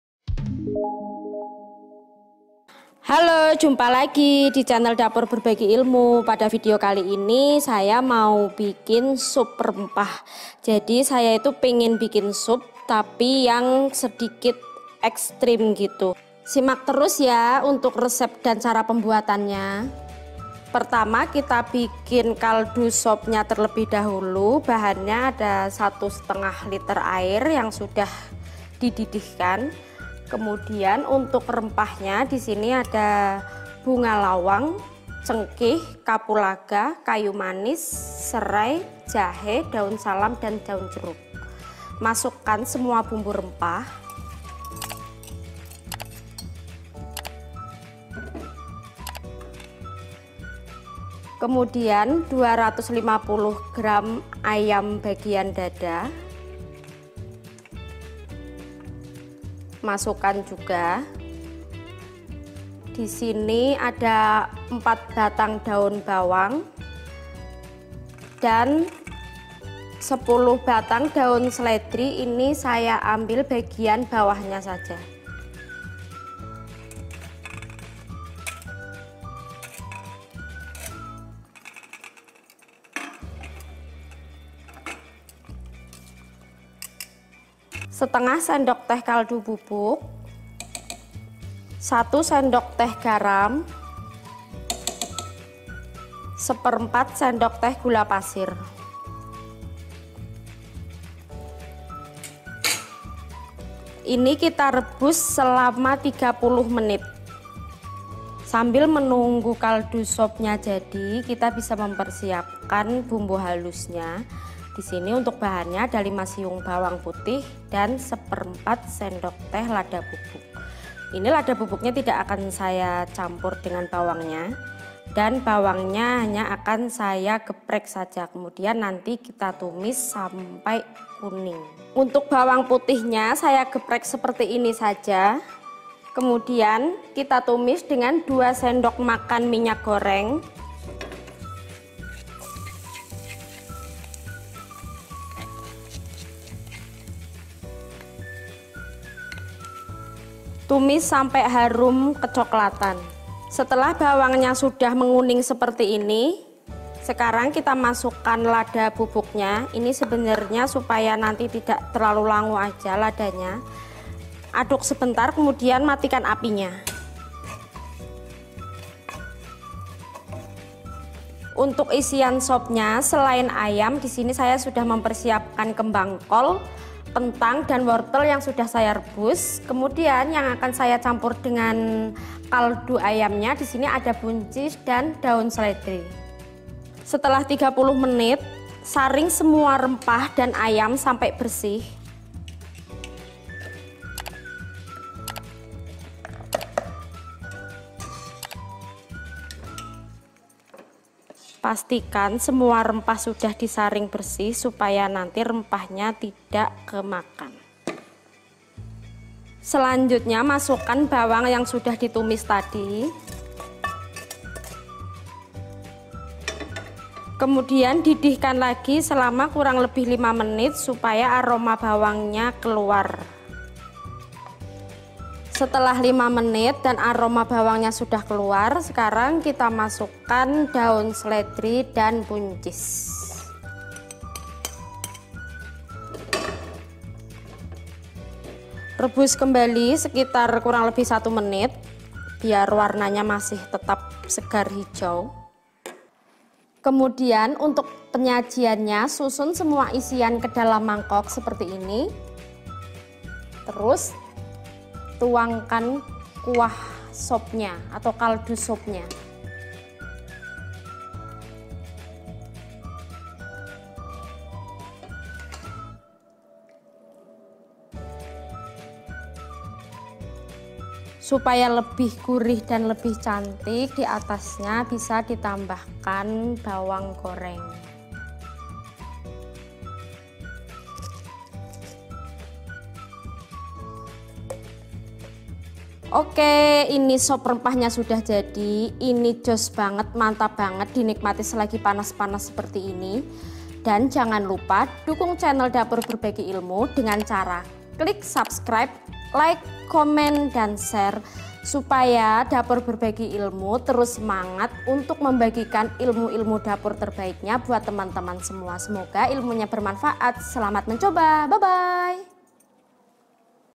RESEP SUP AYAM REMPAH Kemepyar!!!
Resep Sup Ayam Rempah

Pada Video kali ini saya mau bikin menu harian Sup Ayam Rempah, ini supnya jos banget dinikmati selagi panas dan bikin tubuh dari hangat karena dari rempahnya, apalagi musim pancaroba seperti ini yang sedang rawan flue menyerang. Berikut resepnya.

Resep:

1500 ml Air
2 batang serai geprek
2 potong jahe geprek
10 butir kapulaga
10 butir cengkih
1 tangkai bunga lawang
5 lembar daun salam
1 potong kayu manis
5 lembar daun jeruk
4 batang daun bawang
10 batang sledri bagian batangnya
250 gr Ayam Filet ( boleh bagian apa aja)
1 sdt Garam
1/2 sdt Kaldu Bubuk
1/4 sdt Gula Pasir

Bumbu Halus:
5 siung bawang geprek
1/4 sdt lada bubuk
2 sdm minyak untuk menumis

Isi Sup:
Daun Sledri
Buncis
Kembang kol
Wortel
Kentang
Bawang goreng untuk taburan

Untuk cara pembuatannya teman-teman bisa tonton videonya sampai selesai ya...

Selamat mencobaaa
Good Luck teman-teman .

Tonton juga video-video Dapur Berbagi Ilmu yang lainnya ya.